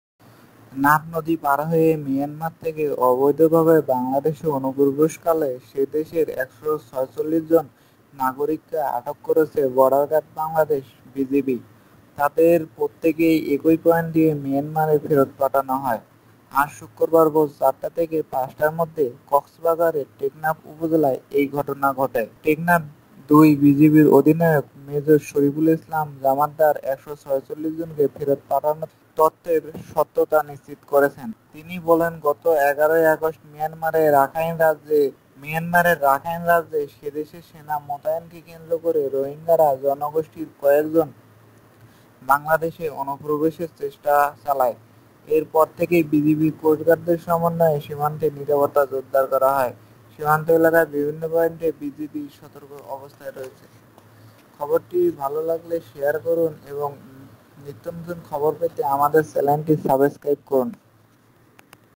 বাংলাদেশে অনুপ্রবেশকালে সে দেশের ১৪৬ জন নাগরিককে আটক করেছে বর্ডার গার্ড বাংলাদেশ
আবারো বাংলাদেশে দলে দলে রহিঙ্গা আসা শুরু করছে, কারন কি কারন?

গত ১১ আগস্ট মিয়ানমারের রাখাইন রাজ্যে সে দেশের সেনা মোতায়েনকে কেন্দ্র করে রোহিঙ্গা জনগোষ্ঠীর কয়েকজন বাংলাদেশে অনুপ্রবেশের চেষ্টা চালায়। এর পর থেকে বিজিবি কোস্টগার্ডের সমন্বয়ে সীমান্তে নিরাপত্তা জোরদার করা হয়। সীমান্ত এলাকার বিভিন্ন পয়েন্টে বিজিবি সতর্ক অবস্থায় রয়েছে।

Rheinga is coming in the team again in Bangladesh, because what causes?

On August 11, some Rohingya people tried to enter Bangladesh in the Rakhine state of Myanmar on the deployment of the army. Since then, security has been strengthened in the border with the BGB Coastguard. BGB is alert in different points of the border area.